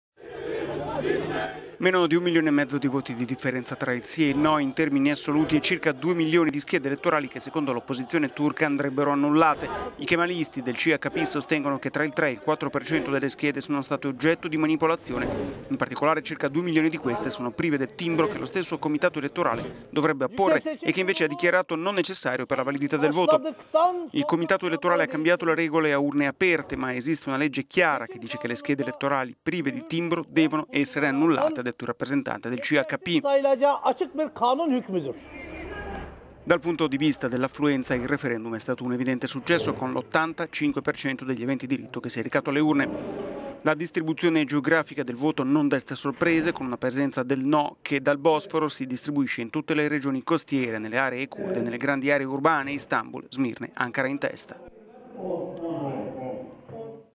Turchia, opposizione contesta 2 milioni di schede e vuole la riconta del 37% dei voti
Meno di 1 milione e mezzo di voti di differenza tra il sì e il no in termini assoluti e circa 2 milioni di schede elettorali che secondo l'opposizione turca andrebbero annullate.

I kemalisti del CHP sostengono che tra il 3% e 4% delle schede sono state oggetto di manipolazione. In particolare circa 2 milioni di queste sono prive del timbro che lo stesso Comitato Elettorale dovrebbe apporre e che invece ha dichiarato non necessario per la validità del voto. Il partito ha annunciato di volere la …